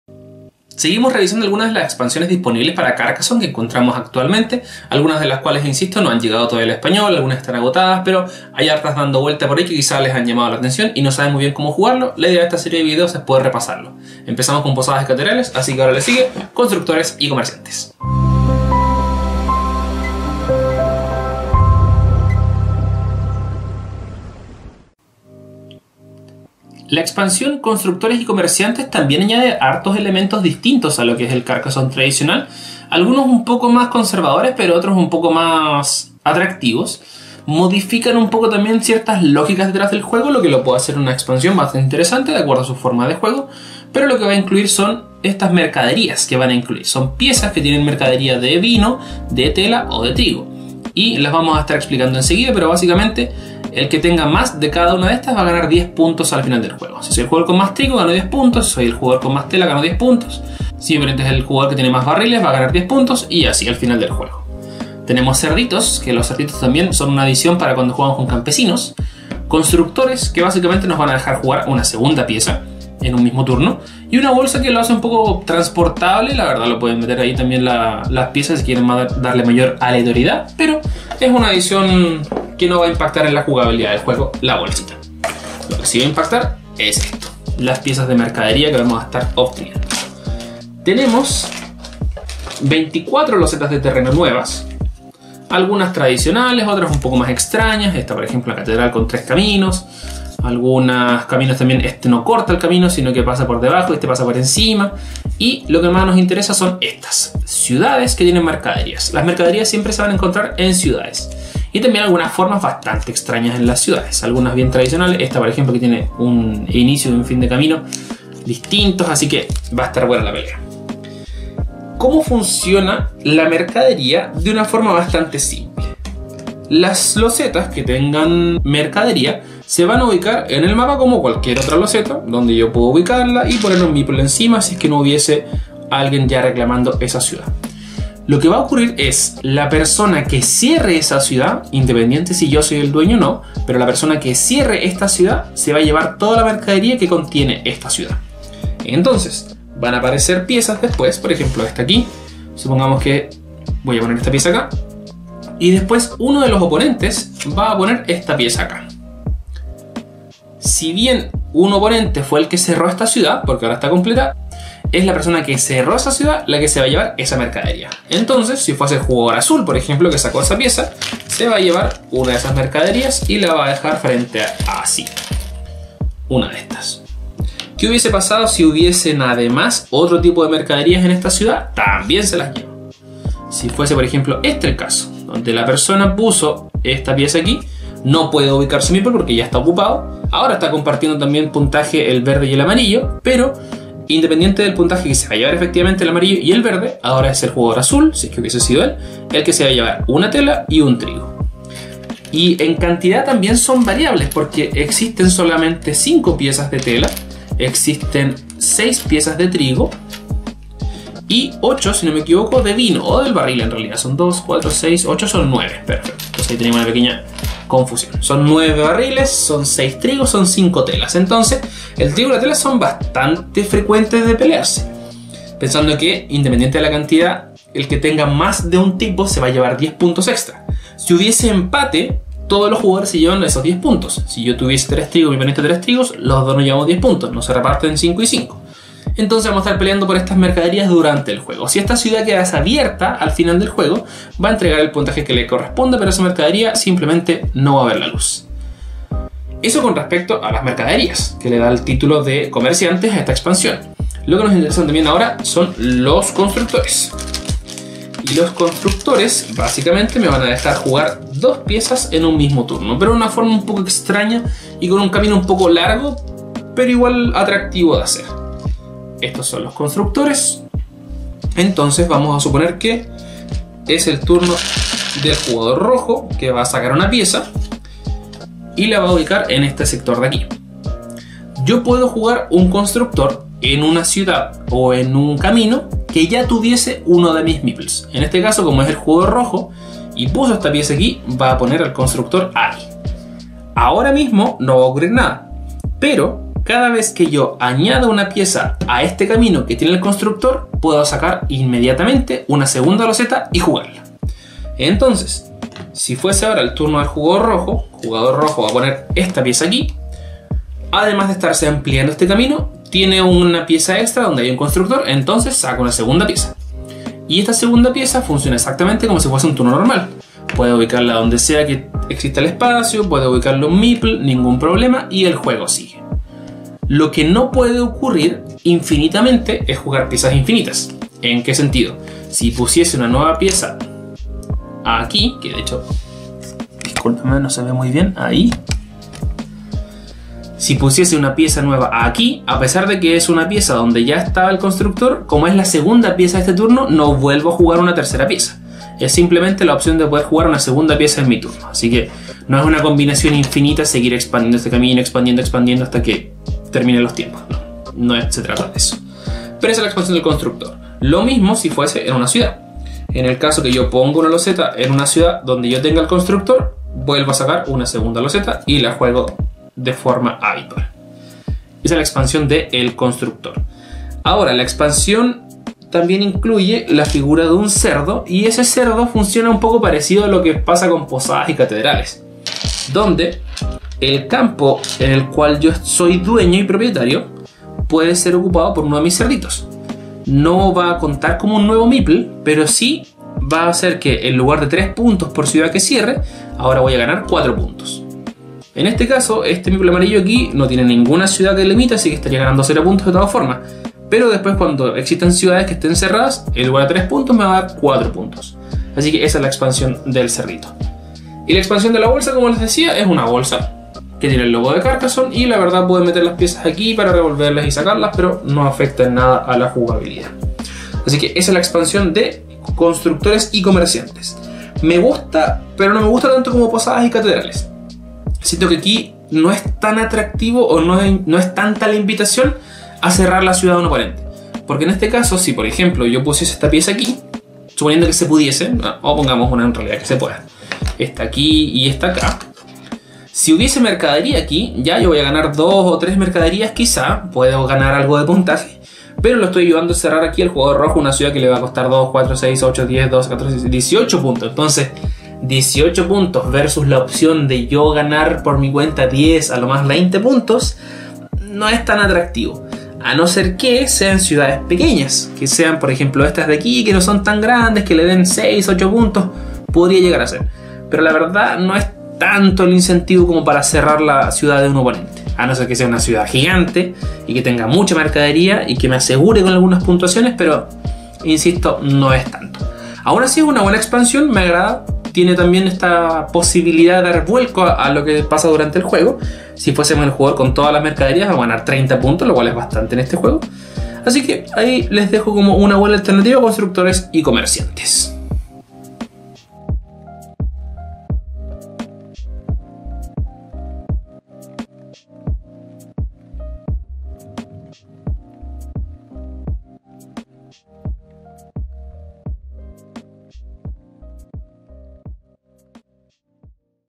Carcassonne Expansión 2: Constructores y Comerciantes - Comentarios y Cómo Jugar
¡Nuevo video en Diluvio Lúdico!

Seguimos con la serie de videos que muestran las diferentes expansiones de Carcassonne, detallando sus diferentes modos de juego, lo que añaden y otras ideas. En esta ocasión llega Constructores y Comerciantes, que dará otro sentido a las ciudades y la posibilidad de ganar algunas mercaderías.

::: Carcassonne Expansión 2: Constructores y Comerciantes :::
Jugadores: 2 - 6
Tiempo: 60 minutos
Edad: 8 años +
Diseñado por: Klaus-Jürgen Wrede
Ilustrado por: Doris Matthäus, Anne Pätzke, Chris Quilliams, Klaus-Jürgen Wrede
Publicado por: Hans im Glück

Espero disfrutes el video.